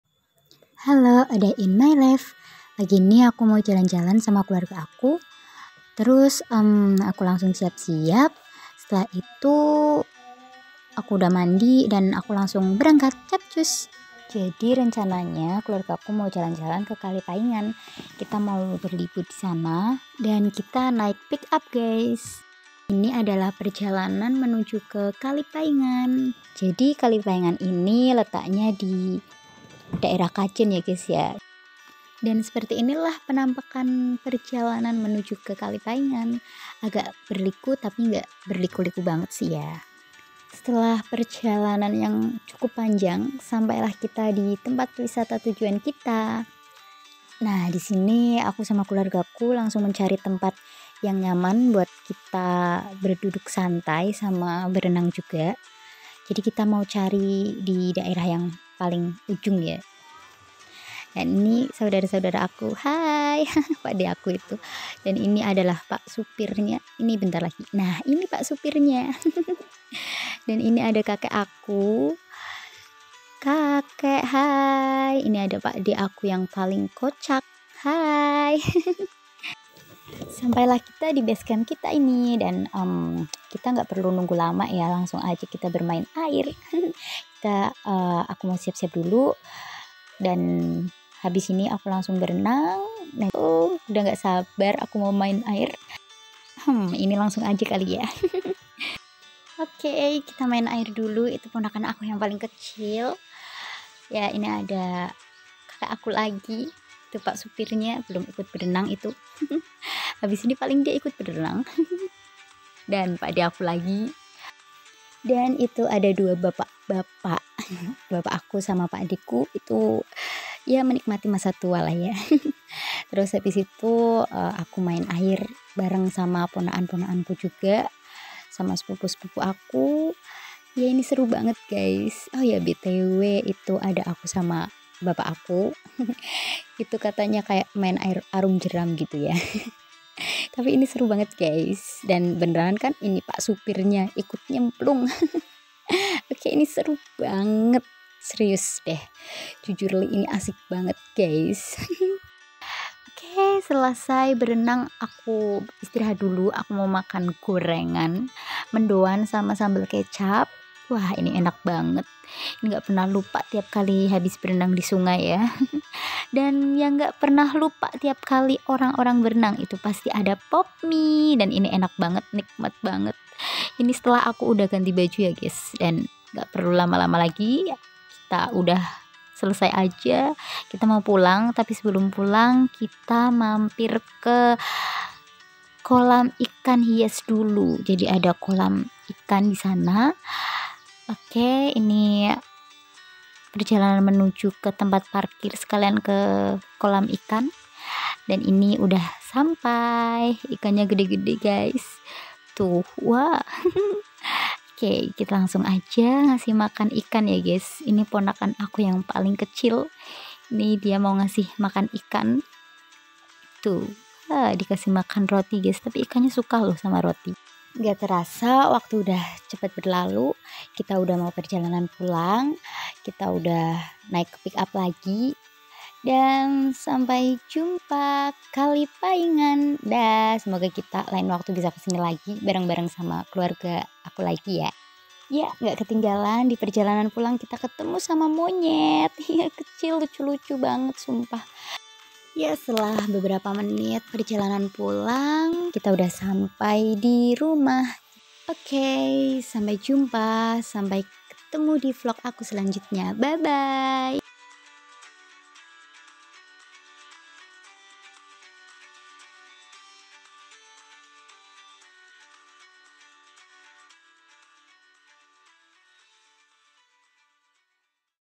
Berwisata ke Kalipaingan, Kajen, Jawa Tengah
Berlibur bersama keluarga di Kalipaingan. Salah satu tempat wisata di Kajen, Pekalongan, Jawa Tengah. Saksikan vlog² aku yang lain juga ya guys...